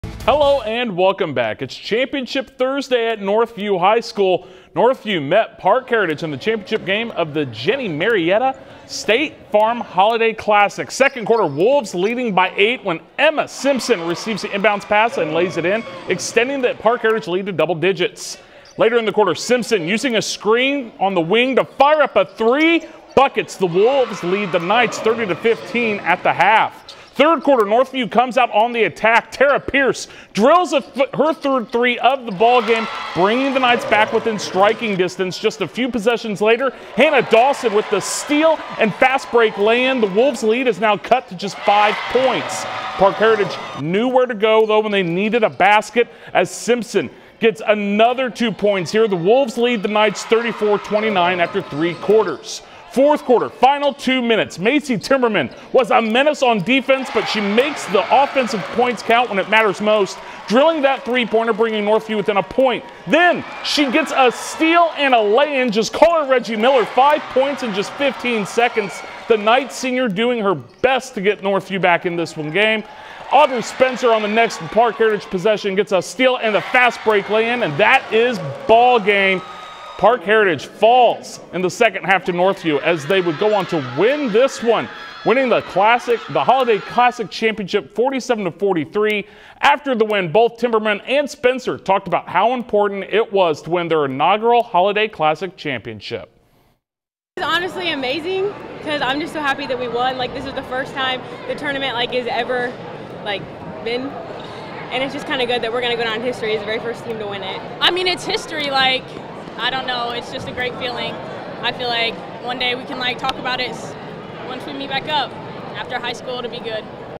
Northview Claims the Jenni Marietta State Farm Holiday Classic Championship
The Lady Knights comeback from being down 15 points at the half to beat the Wolves and win their inaugural holiday classic championship.